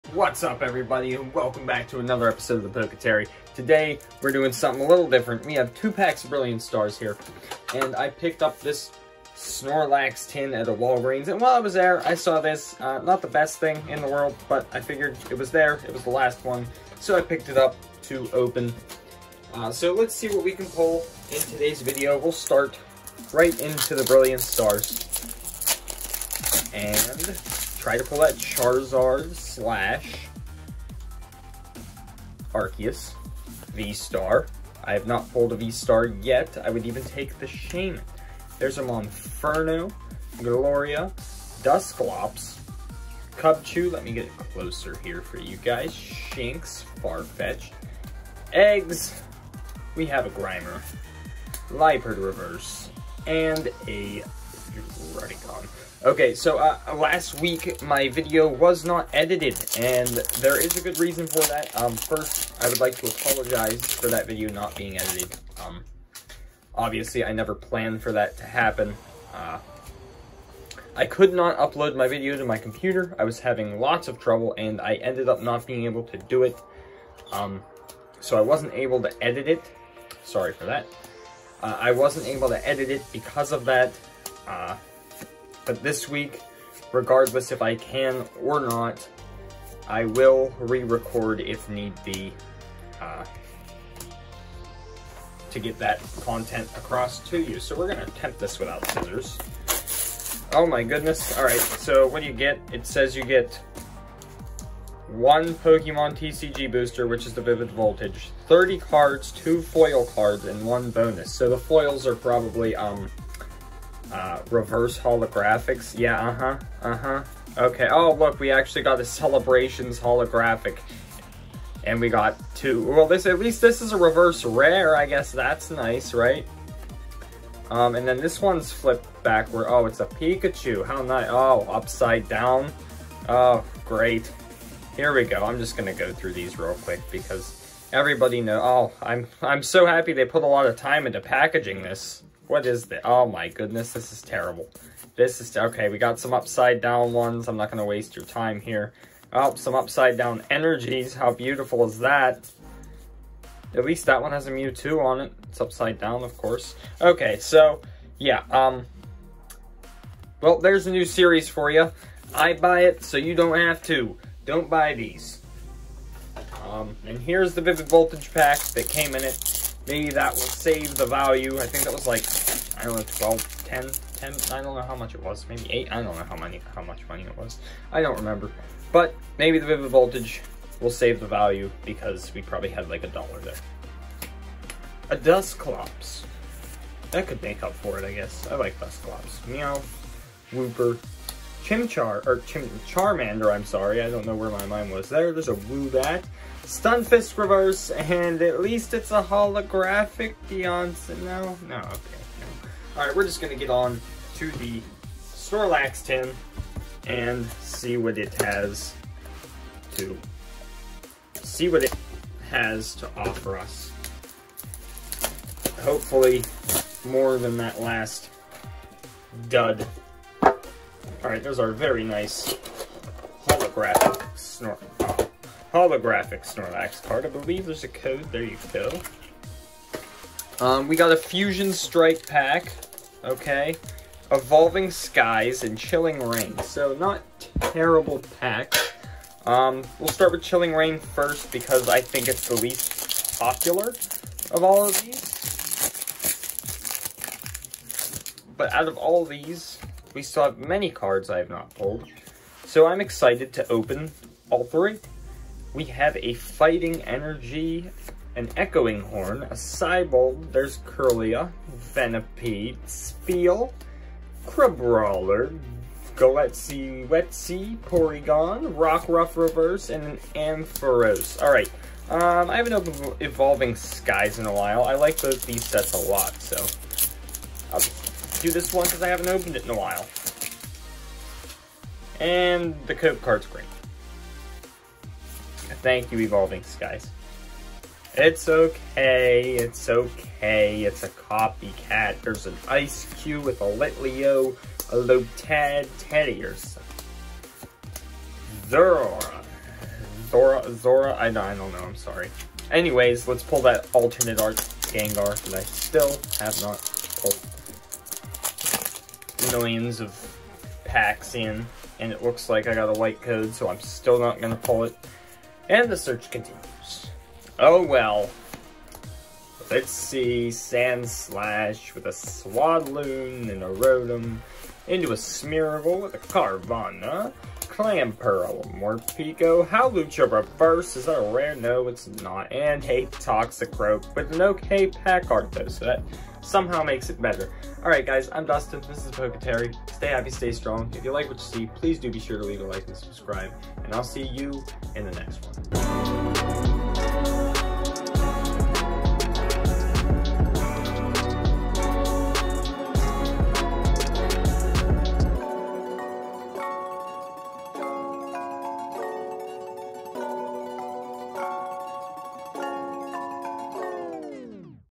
Pokemon mystery blister and snorlax tin opening!
Pokemon mystery blister and snorlax tin opening new series (I open it so you don't have to.)